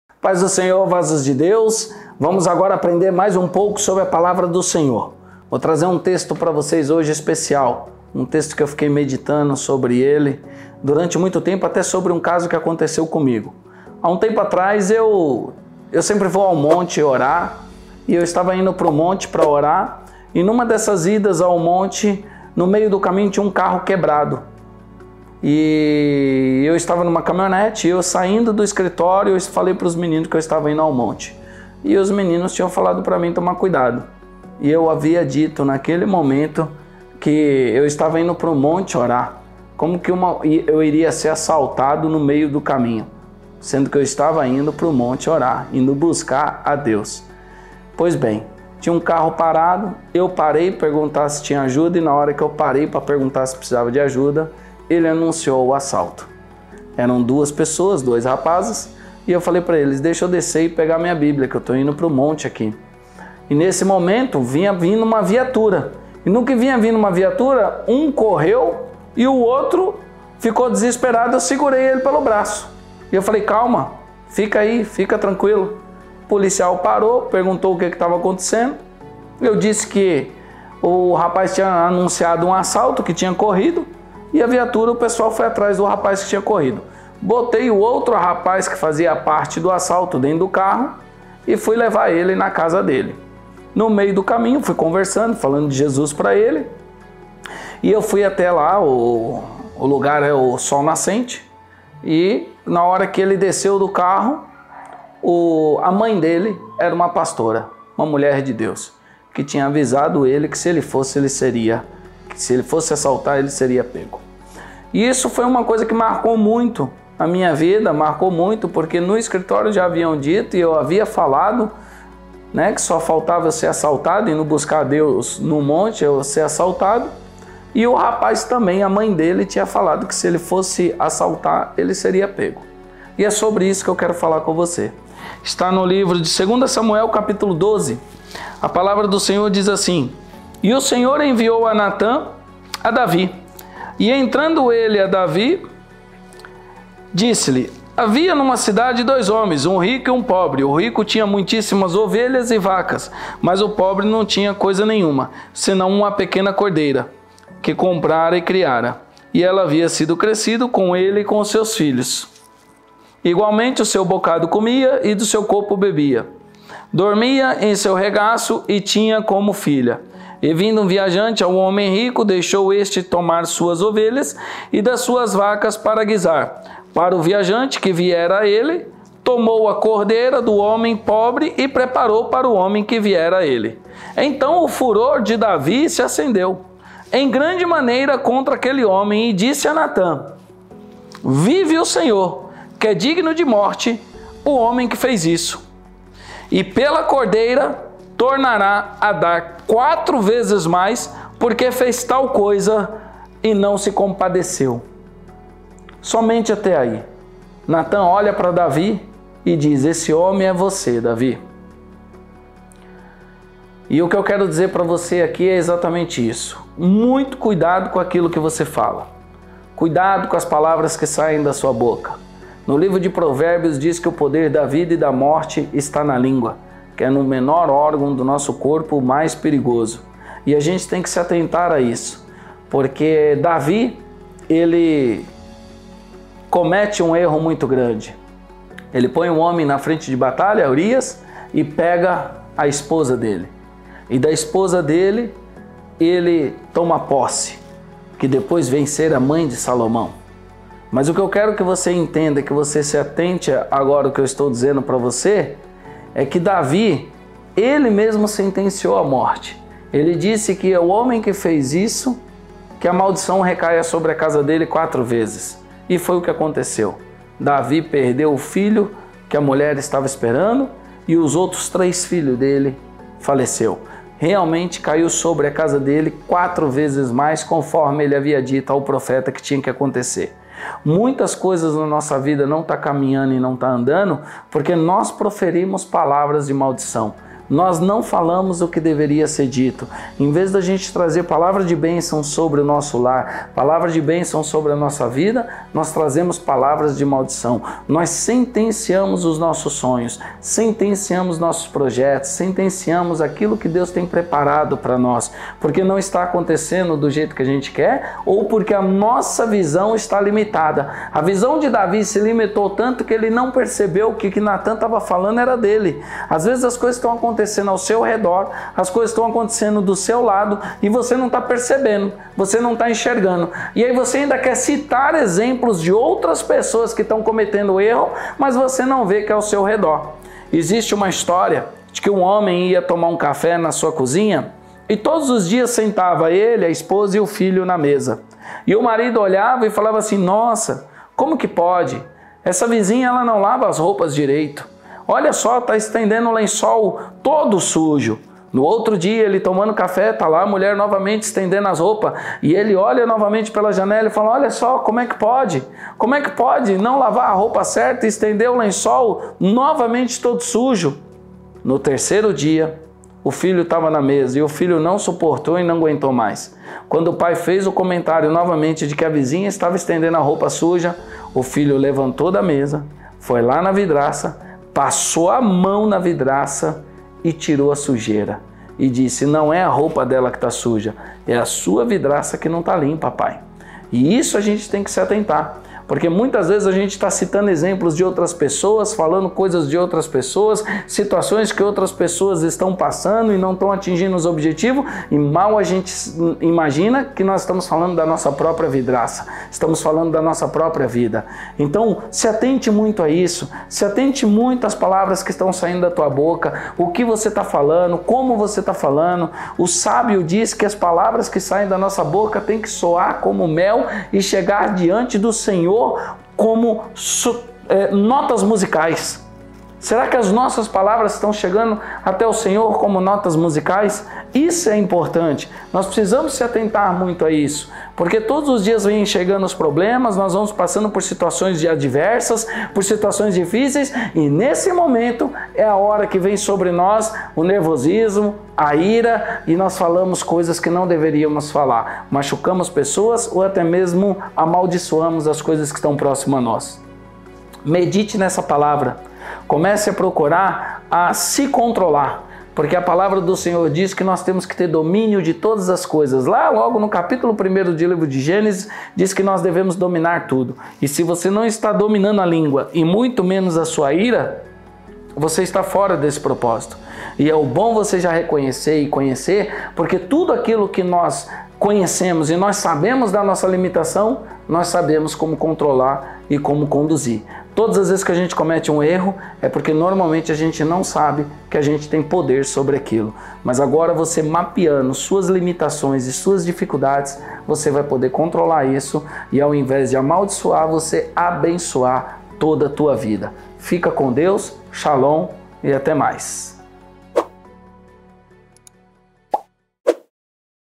Cuidado com as coisas que você fala | Cesar Zanoni
Todos nós precisamos estar vigilantes até mesmo com a palavra que sai da nossa boca. A bíblia nos ensina que o poder da vida e da morte está na língua.  O que tem saido da sua boca? São palavras de bençãos ou de maldição?

• Converse com a Assessoria Pr. Cesar Zanoni